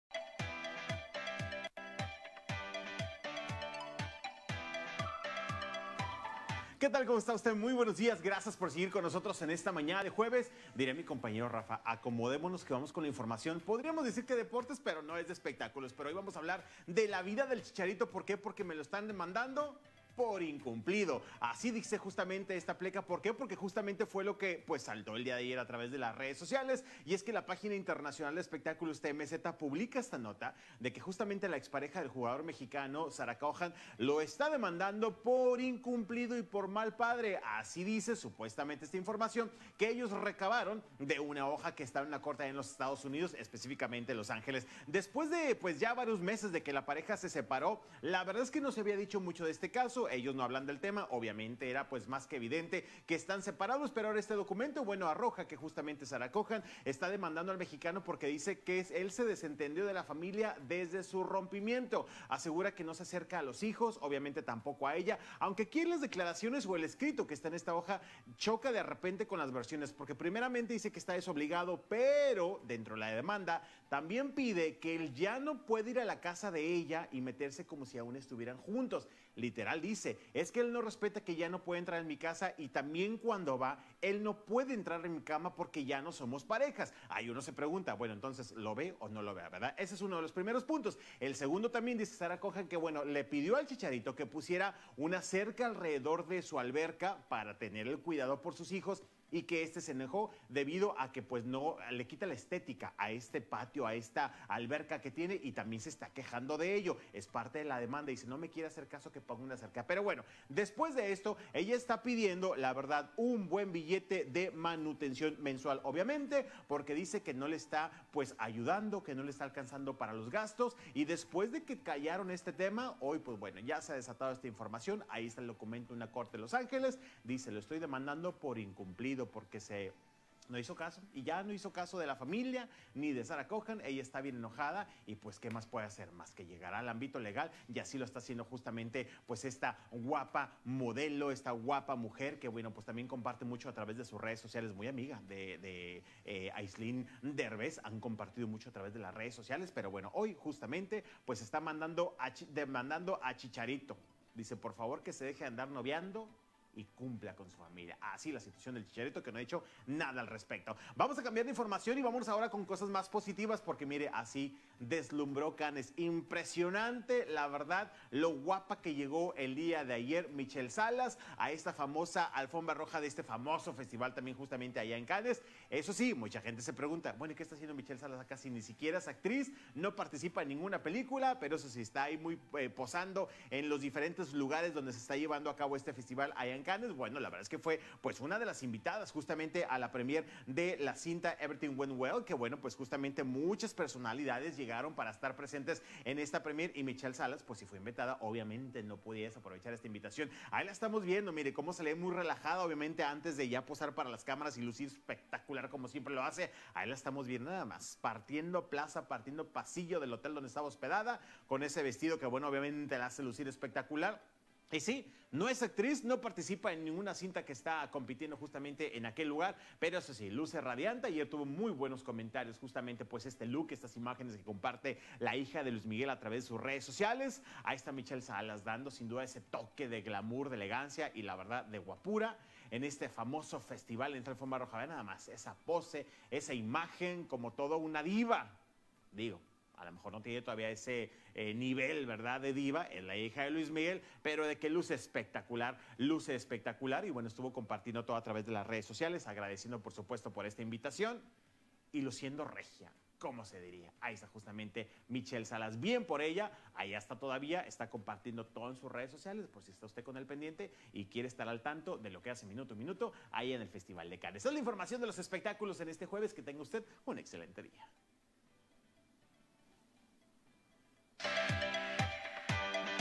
Demandan a Chicharito por incumplido
En los espectáculos, demandan ex pareja al chicharito por incumplido y mal padre; además Michelle Salas deslumbra en Cannes. Entérate de las noticias más impactantes del mundo de los espectáculos.